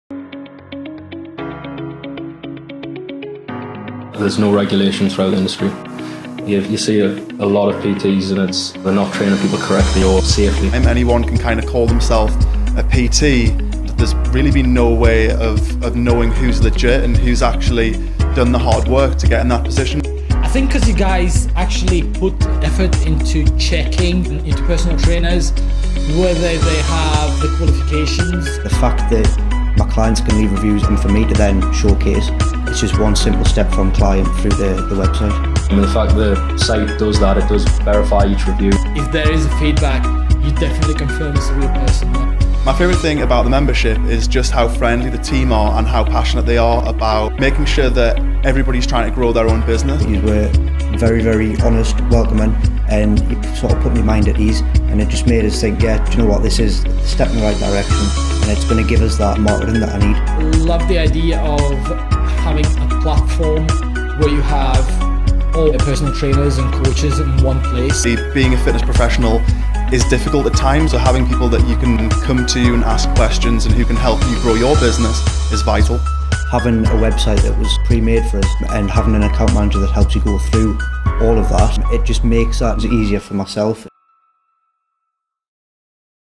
Why Joining Fitbook is so Important!
In this video our members tell us the reasons behind why Fitbook is so important for the health & fitness industry and how it helps their business.